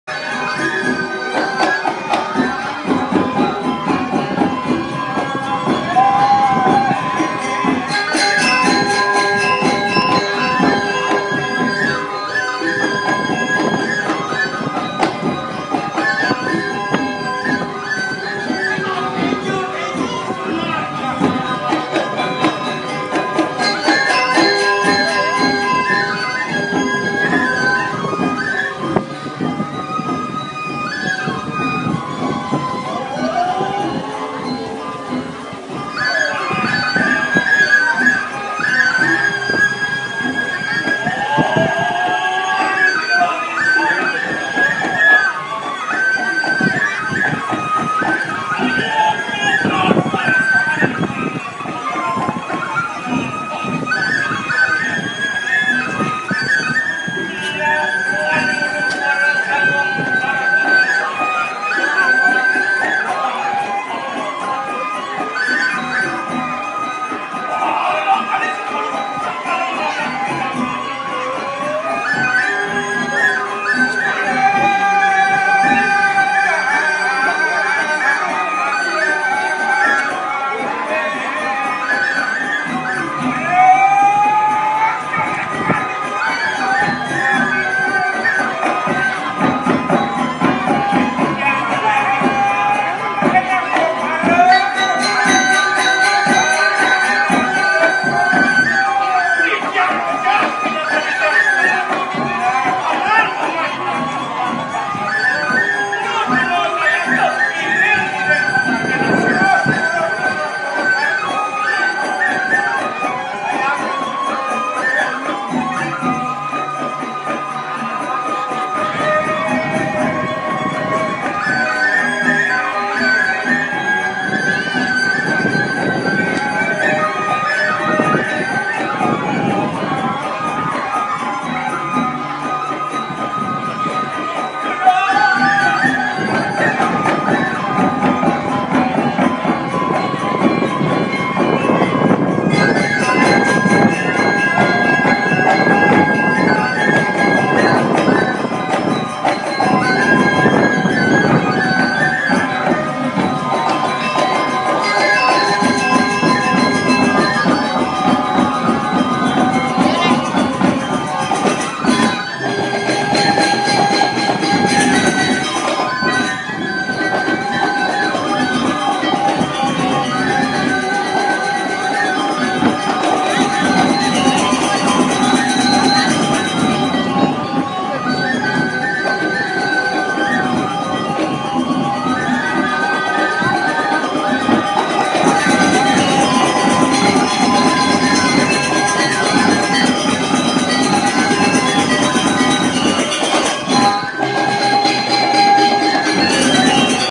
Balinese Traditional Theater during Bali Arts Festival 2014
I manage witness this amazing Balinese Traditional theater accompanied by live Balinese Gamelan at the Bali Arts Festival 2014.